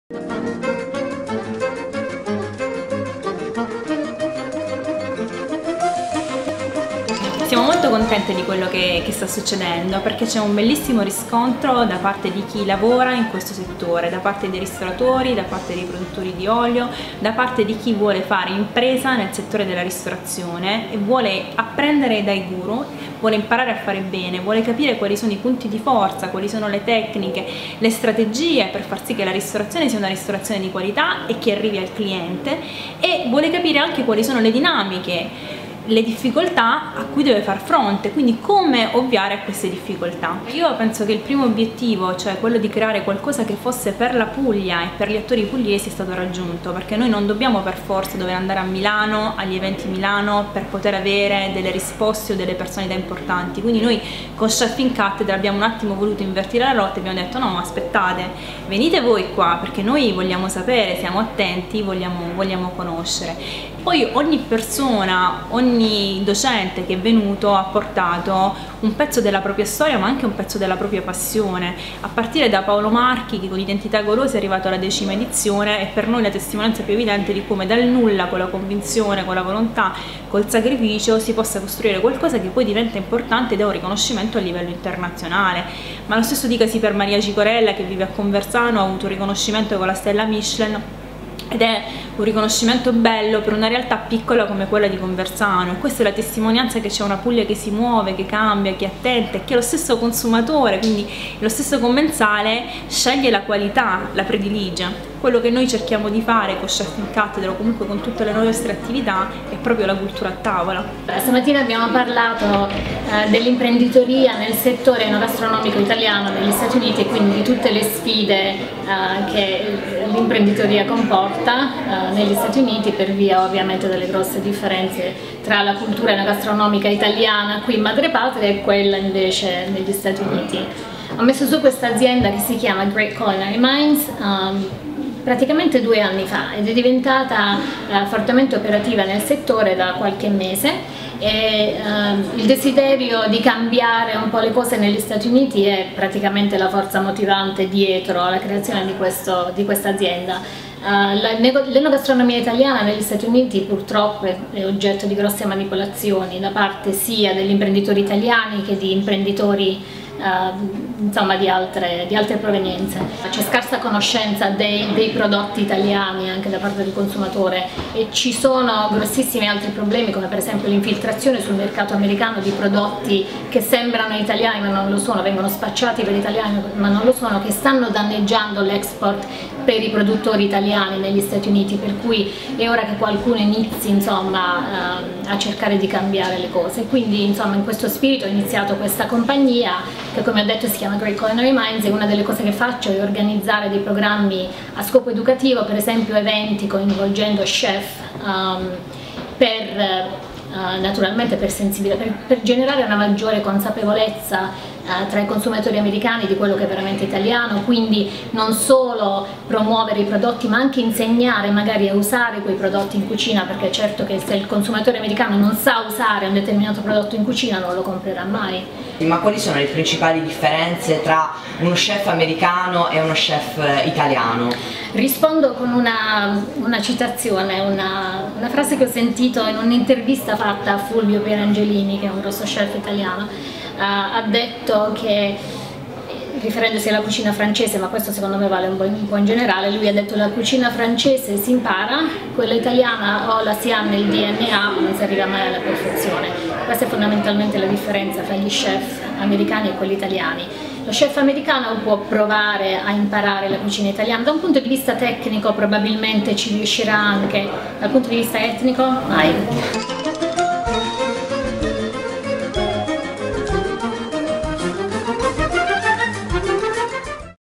LdbStorytelling_Chef in Cattedra
Ursula Avella e la cucina italiana all'estero.
Guagnano (LE) 20/05/2014